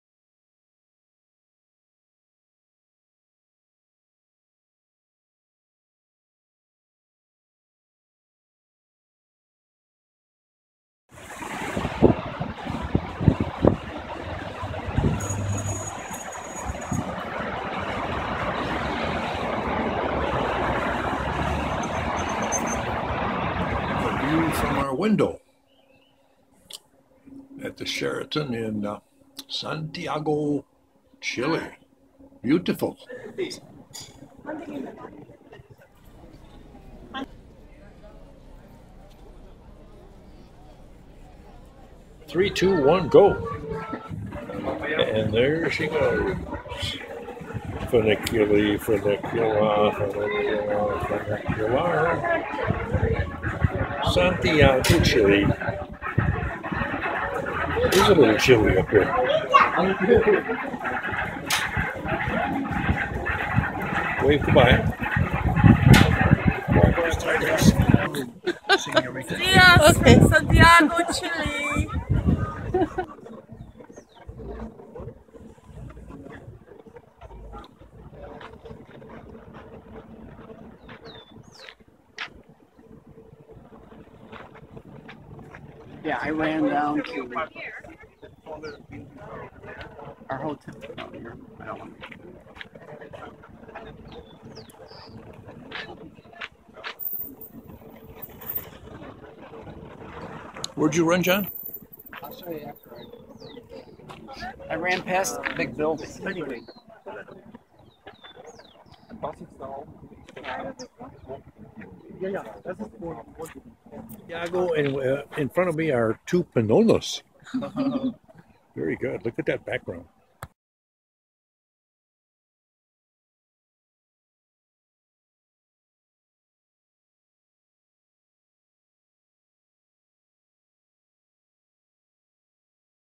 SantiagoChile2014
Santiago, Chile, 2014, with John & Diane on a 14 day cruise around Cape Horn...